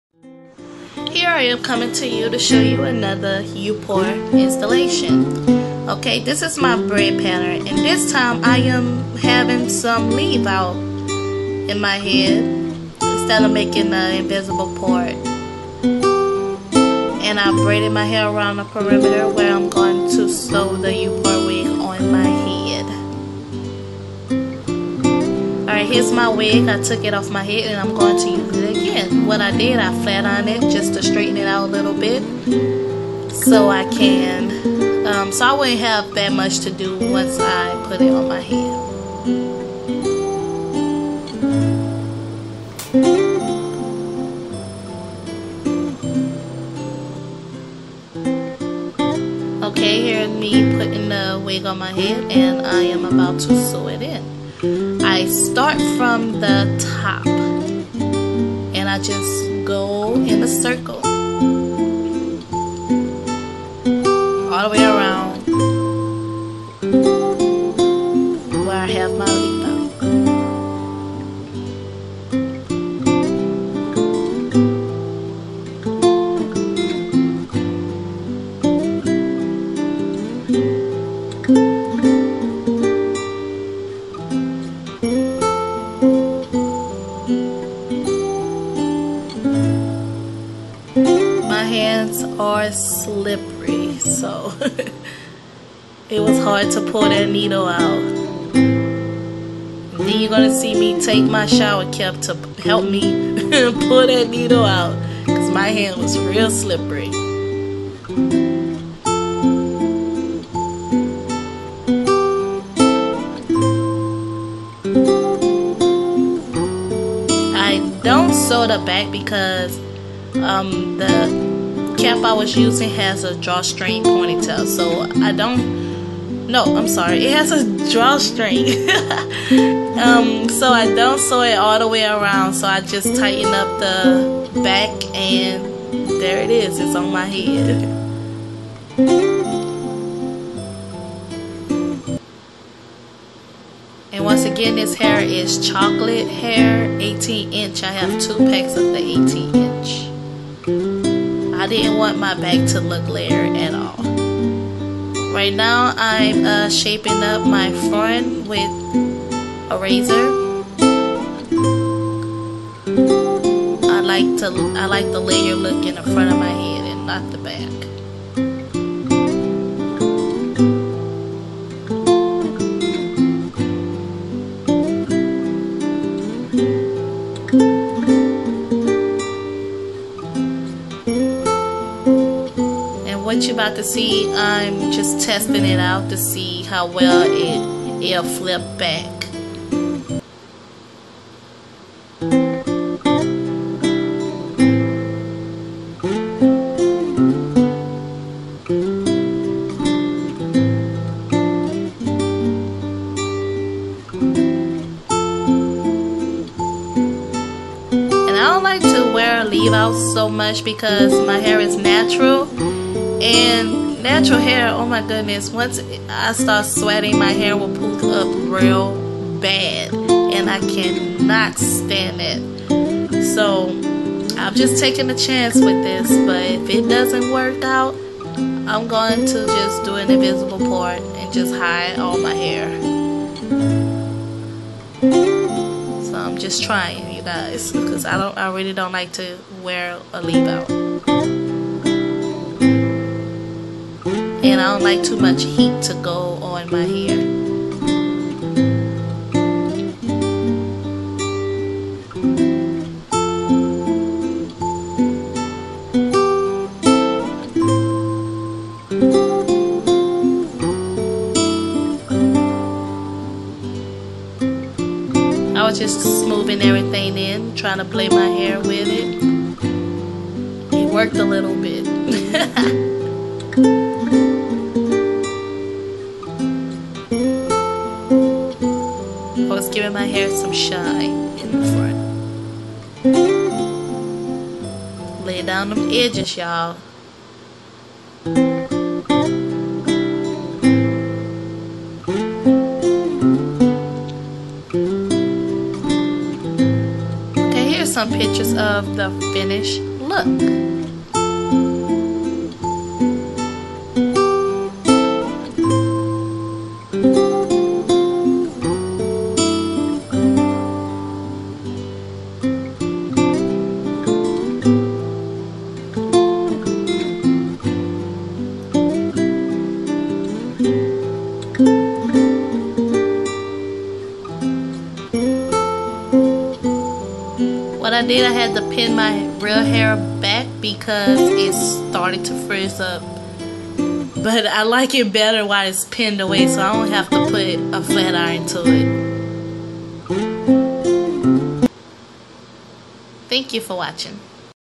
U-Part Wig (QUICK WEAVE)
In this video I'm showing how to put on the u part wig